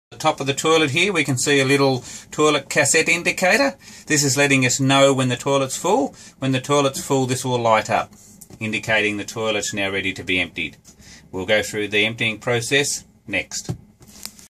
Montana Caravan: Toilet Empty Warning Light
This will show you how to know when the toilet cassette is to be emptied.